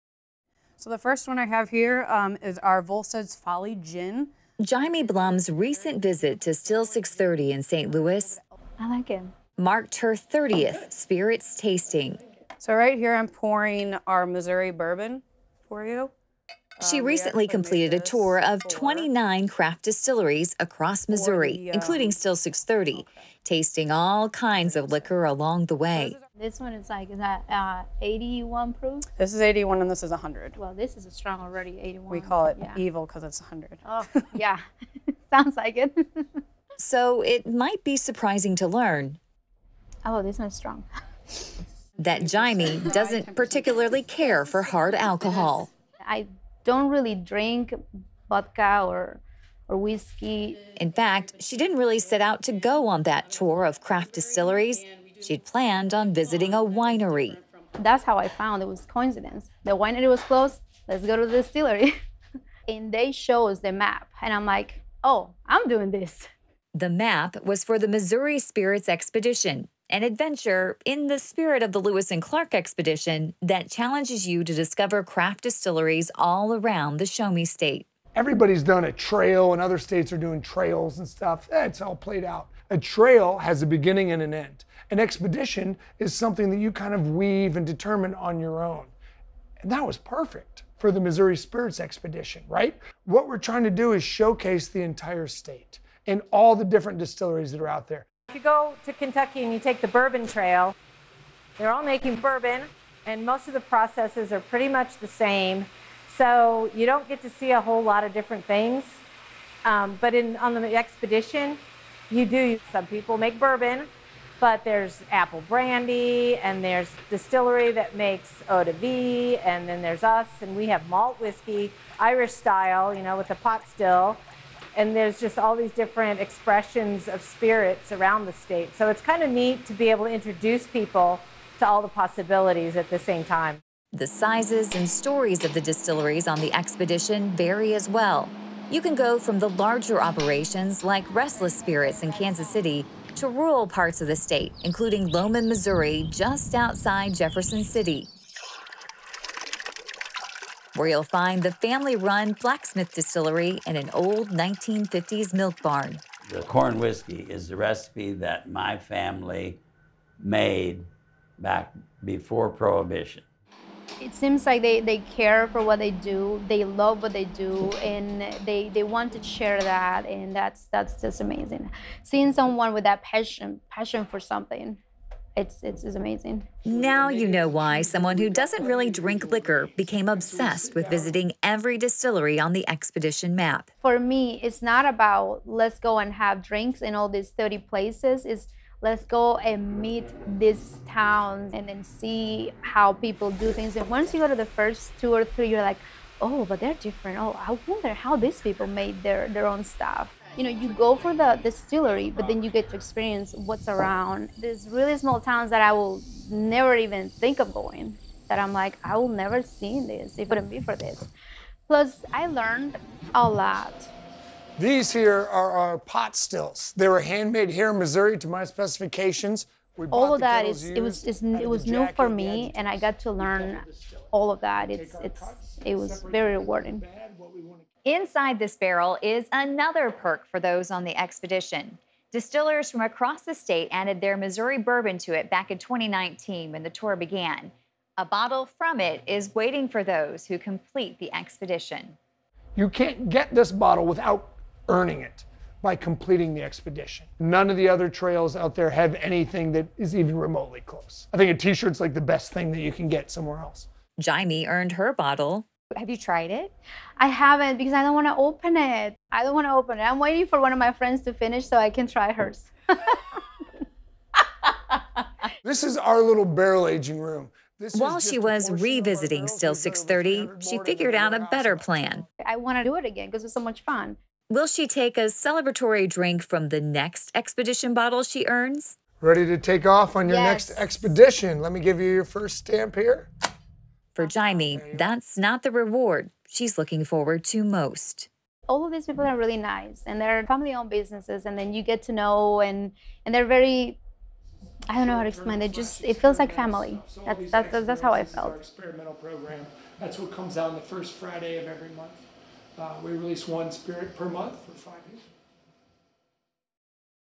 Missouri Spirits Expedition: One Woman’s Journey to Complete Missouri’s Craft Distillery Adventure
The Missouri Spirits Expedition is a trail of craft distilleries all around the Show-Me State. Unlike similar trails in Kentucky or Tennessee, it features a variety of handcrafted spirits from bourbon and whiskey to brandy and gin. We talk with Yaimairis Blum who earned several perks by completing the expedition including a free bottle of exclusive Missouri Bourbon. But, she also found the experience of learning about the craft of distilling spirits, discovering towns across Missouri and meeting people passionate about their jobs was just as rewarding.